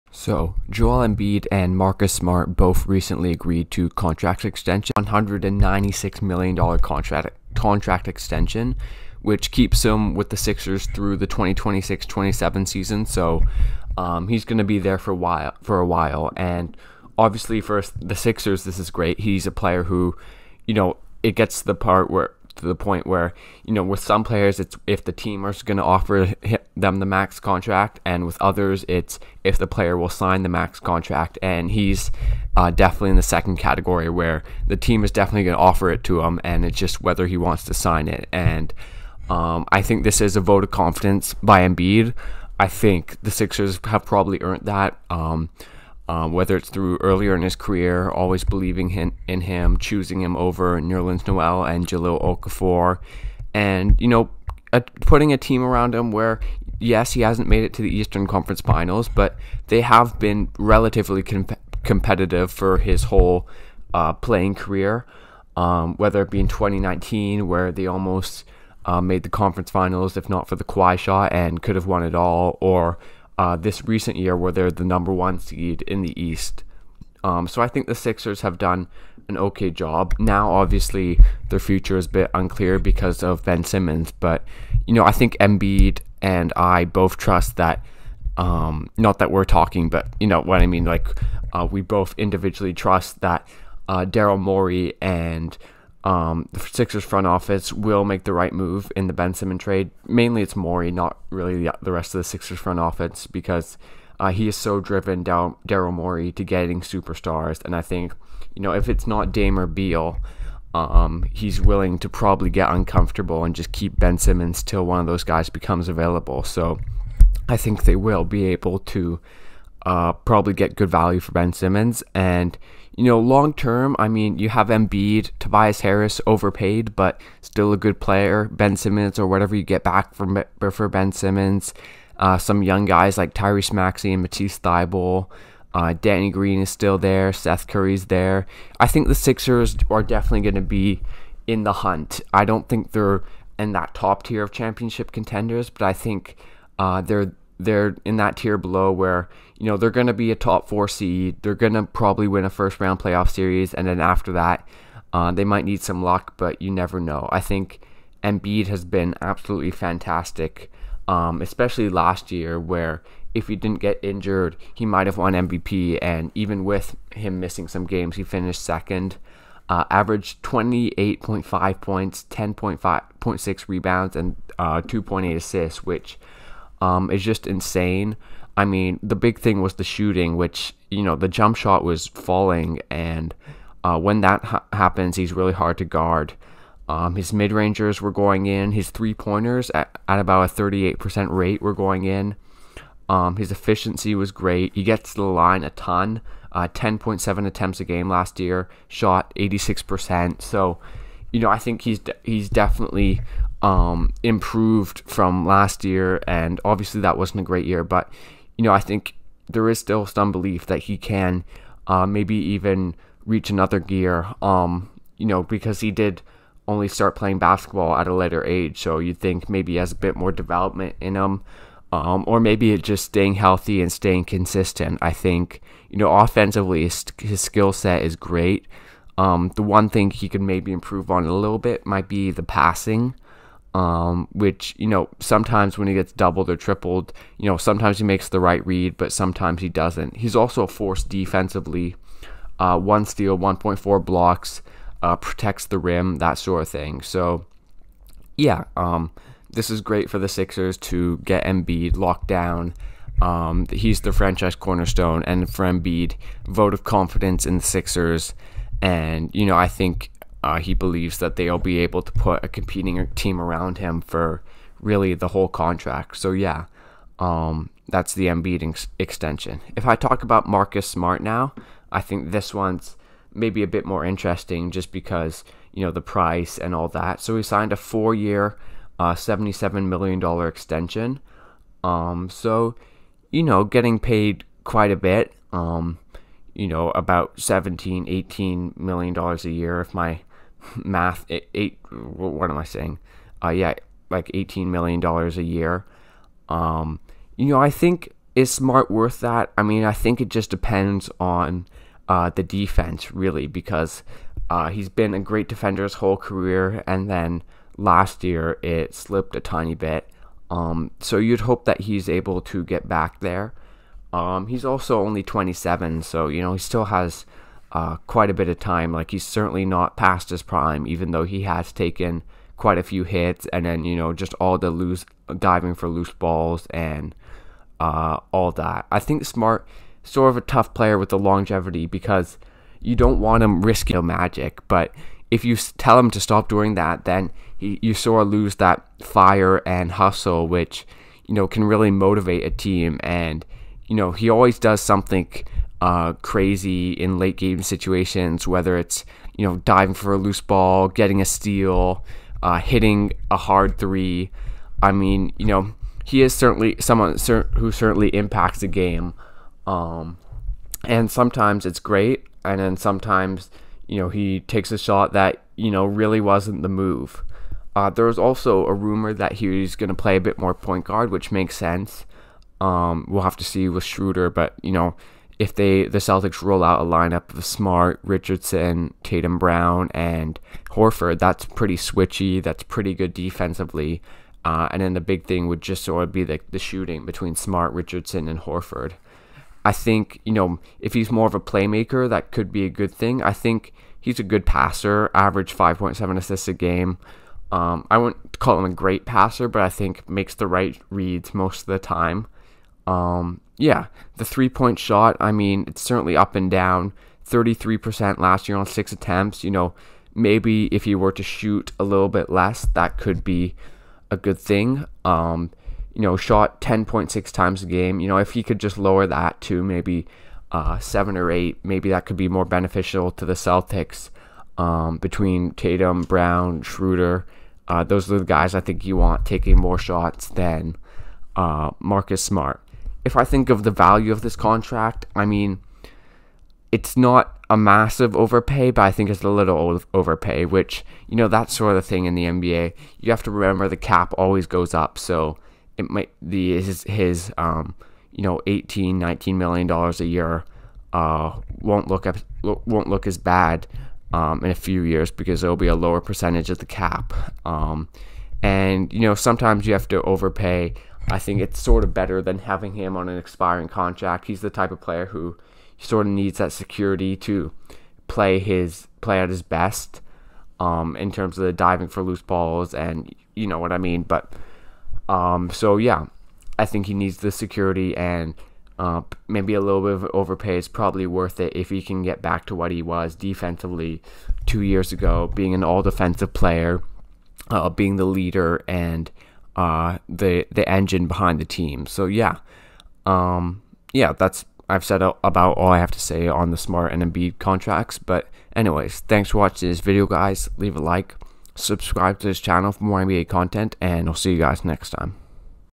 Joel Embiid Signs Supermax Extension With Sixers
I also talk about the Celtics extending Marcus Smart